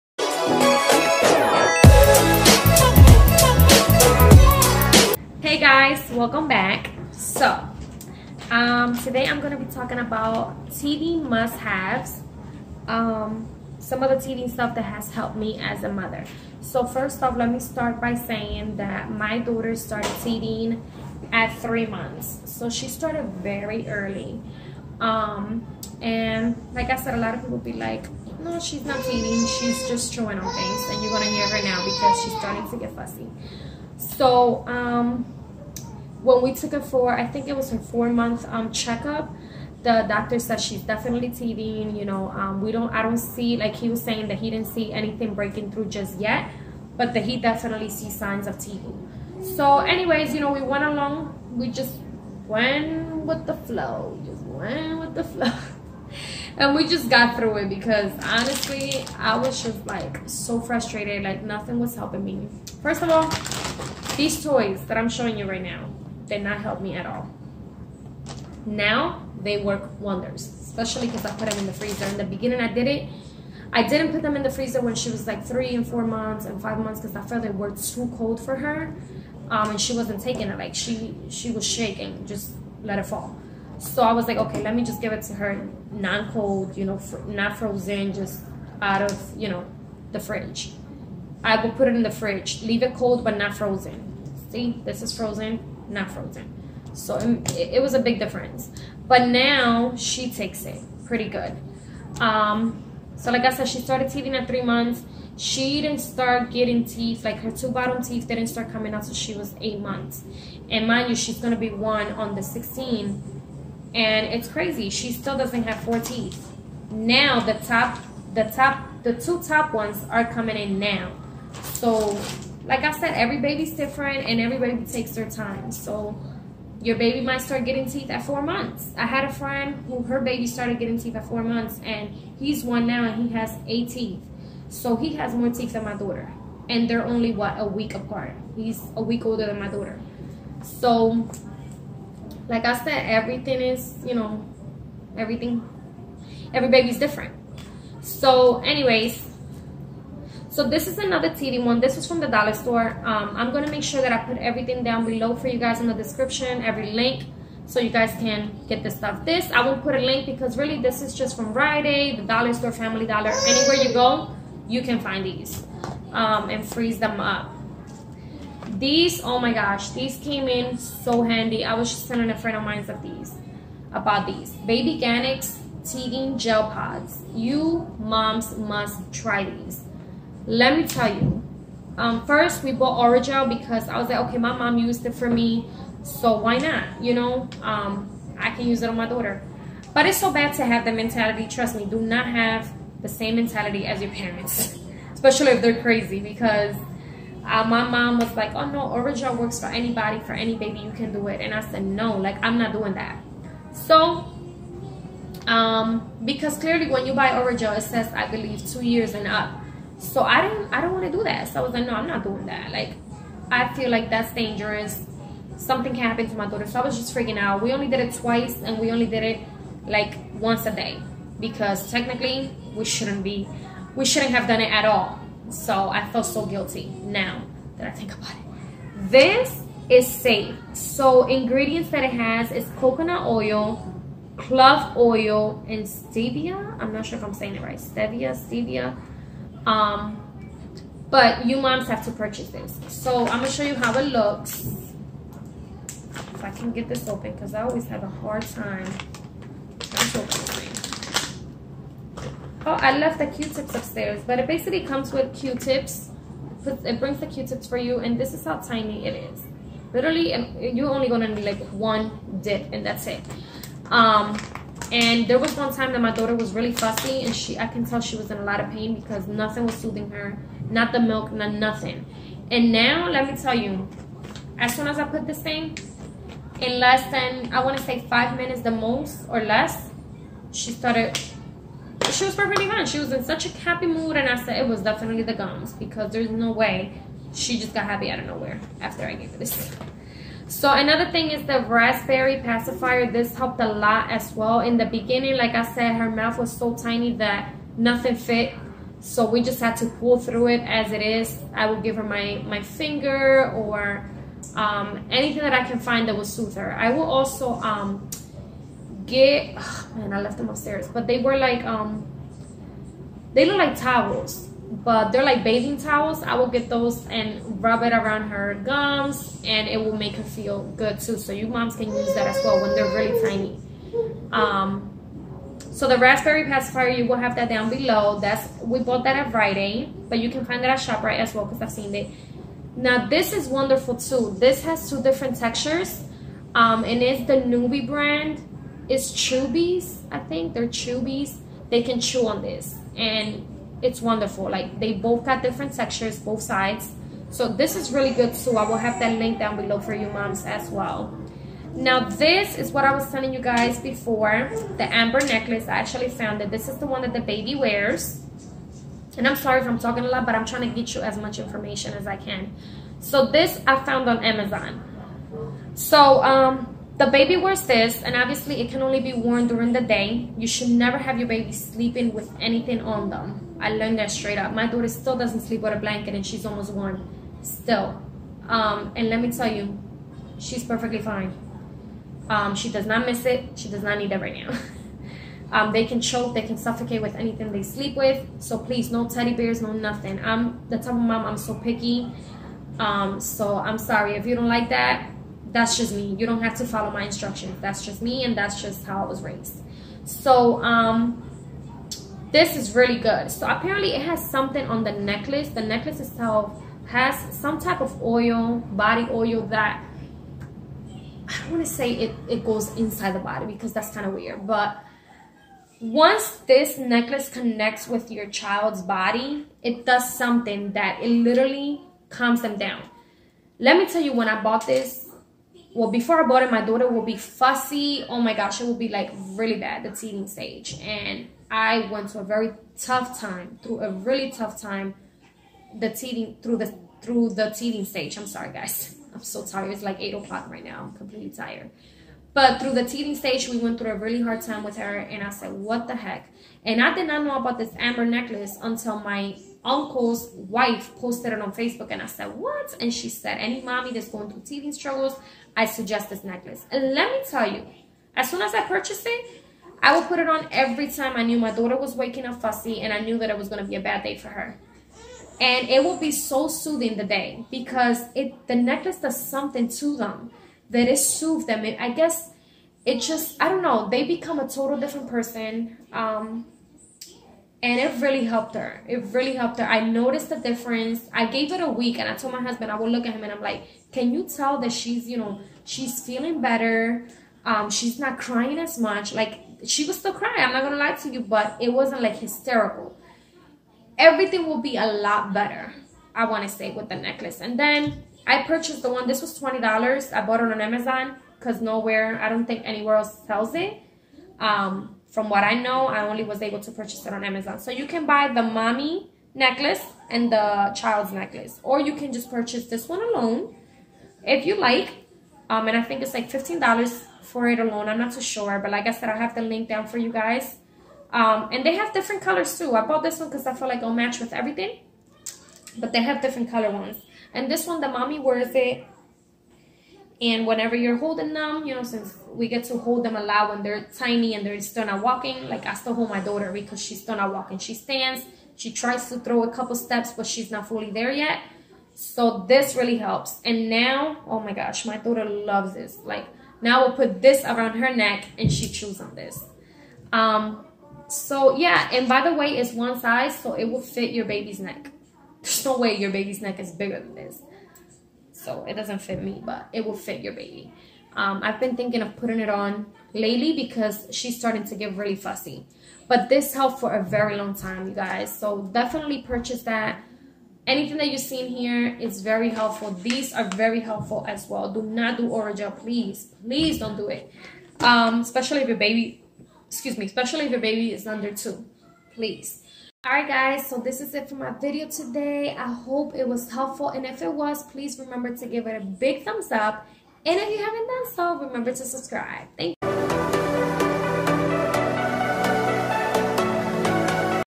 Teething MUST HAVES 👍
Hey ya'll, this video is me telling you the top teething essentials that helped my daughter during teething stage. Still helps her. Links are right here below.

Nuby Pacifier:

Raspberry pacifier:

Babyganics Teething gel pods:

Amber teething necklace:

So those are the links guys, give this video a BIG thumbs up if it was helpful. xoxo!